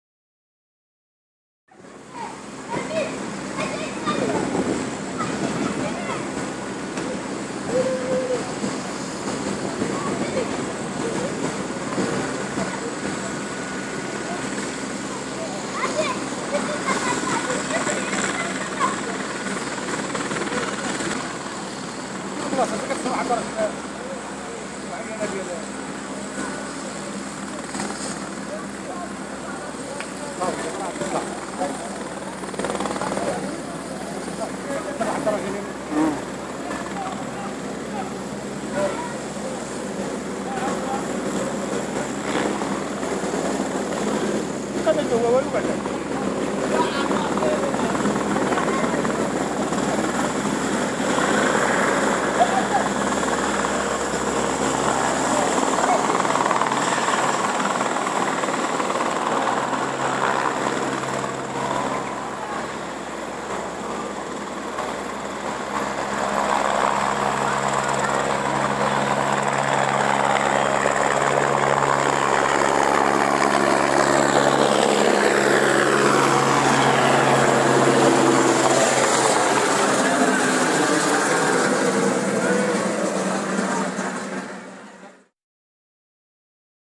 إقلاع طائرة هيلكوبتر من الملعب البلدي لوزان
إقلاع طائرة هيلكوبتر من الملعب البلدي لوزان لتصوير المرحلة الثالثة للدوري الدولي الخامس لسباق الدراجات التي انطلقت من مدينة وزان.